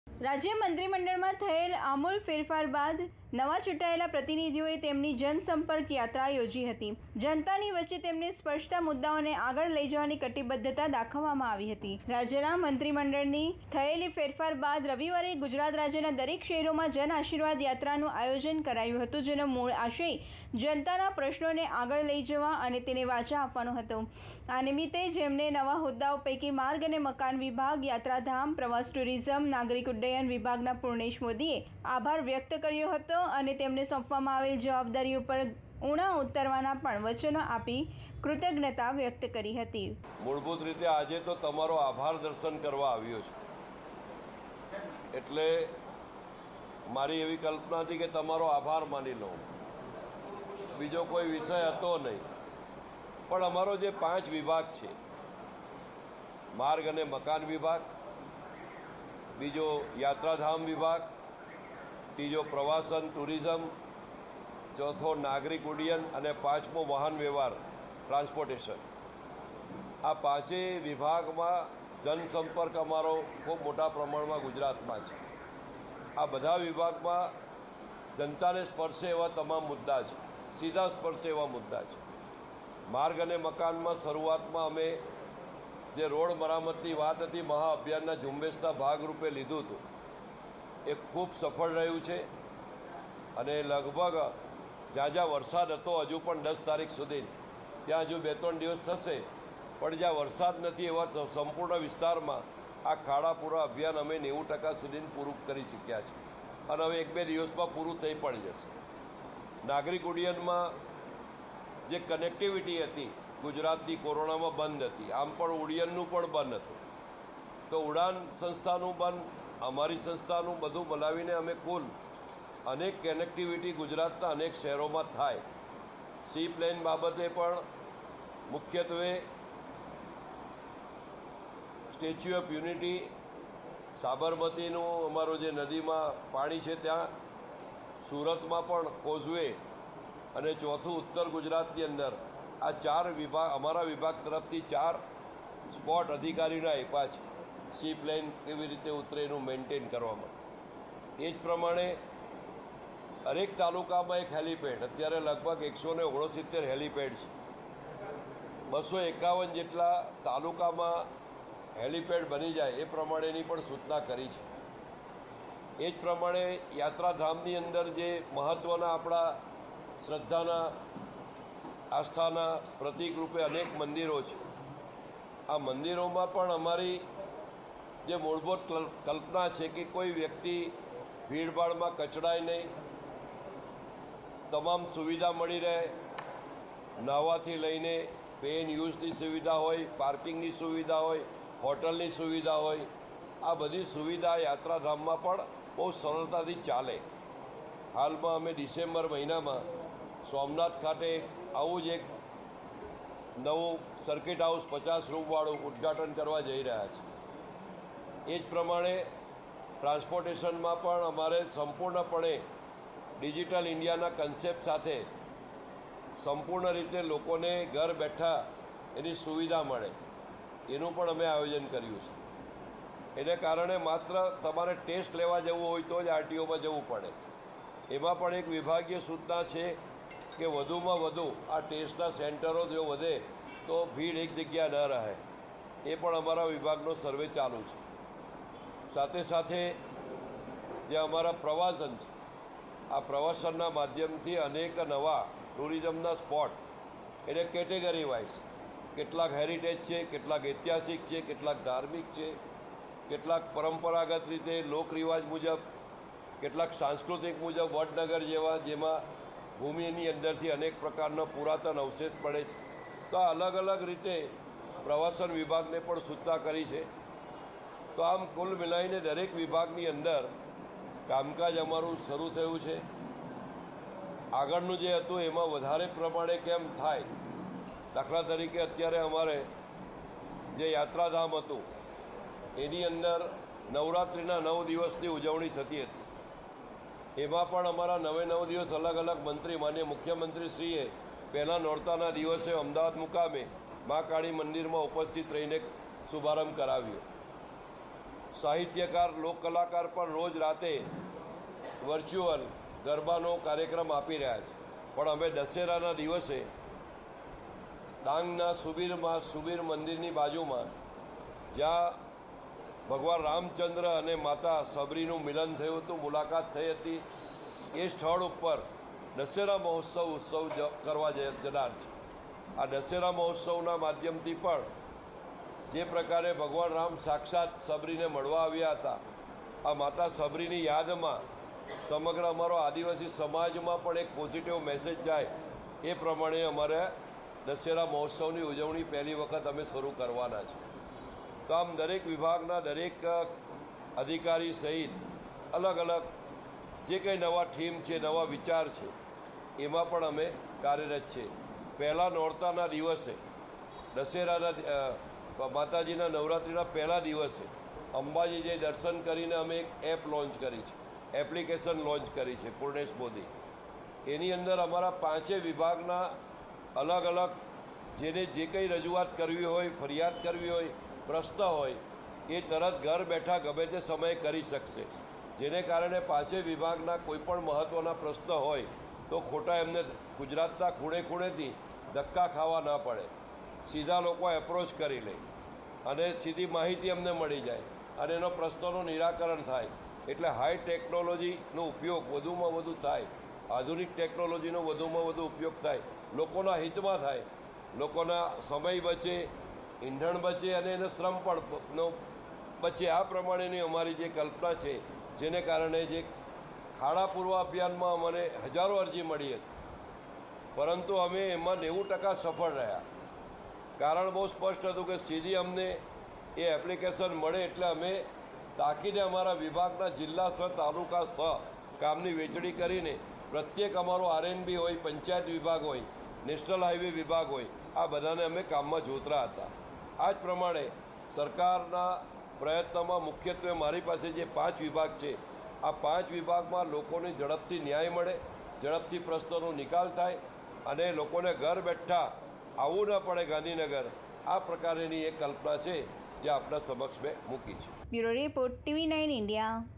જન આશીર્વાદ યાત્રાનો પ્રતિસાદ ખુબ સારો રહ્યો ......|TV9INDIA GUJARATI
રાજ્યના મઁત્રી મંડળ માં થયેલ ફેરફાર બાદ રવિવારે ગુજરાત રાજ્યના દરેક શહેરોમાં જન આશીર્વાદ યાત્રાનું આયોજન કરવામાં આવ્યું હતું.જેનો મૂળ આશય જનતાના પ્રશ્નોને આગળ લઇ જવા અને તેનો વાચા આપવાનો હતો.આ નિમત્તે જેમને નવા હોદાઓ પૈકી માર્ગ અને મકાન વિભાગ,યાત્રા ધામ,પ્રવાસ ટુરિઝમ,નાગરિક ઉડયાયન વિભાગના પૂર્ણેશ મોદીએ આ બાદલ આભાર વ્યક્ત કર્યો હતો અને તેમને સોંપવામાં આવેલ જાવાબદારી ઉપર  ઉણા ઉતારવાના પણ વચનો આપી કૃતગ્યાતા વ્યક્ત કરી હતી...